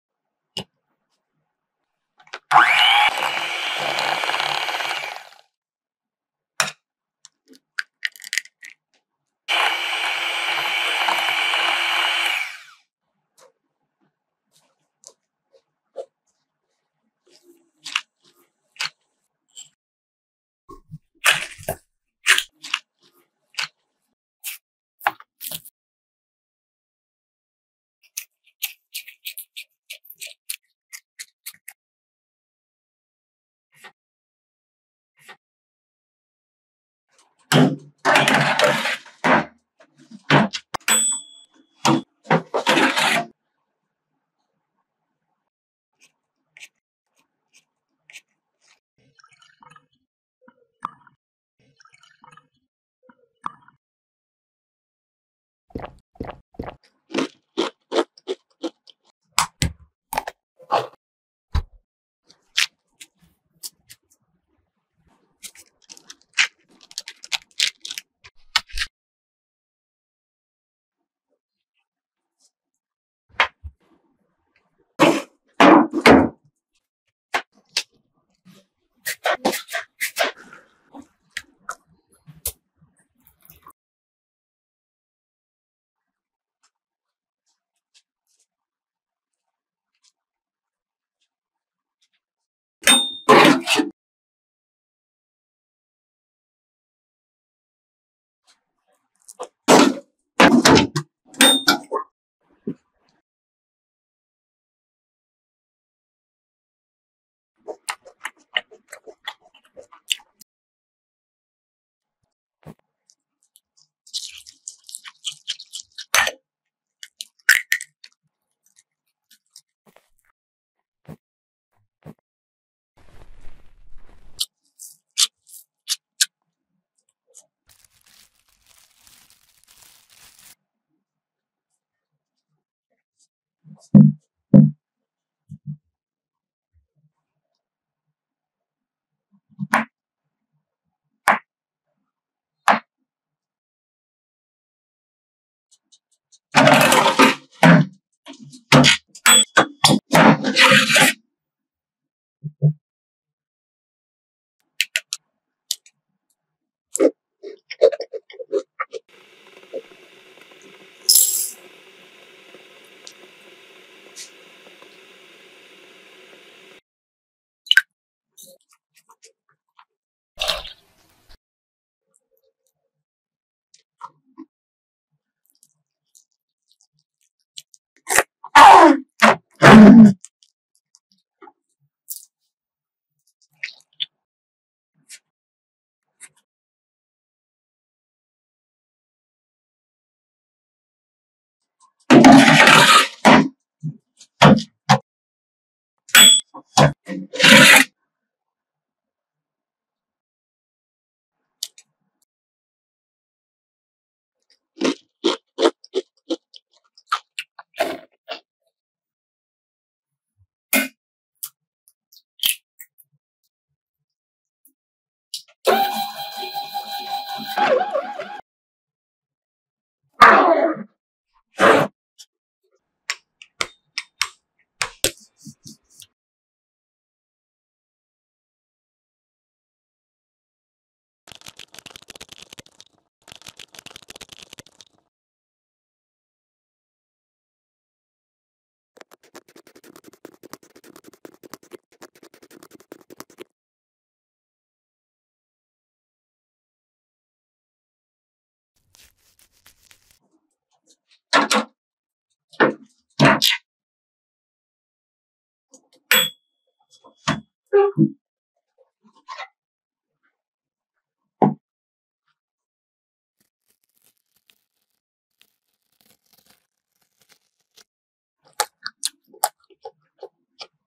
Tiktok ASMR Cooking 🍫 [Engsub] Easy Cooking Recipes Of Some Easy-To-Make Fruit Desserts Cakes
Our content is made possible by RubyCake.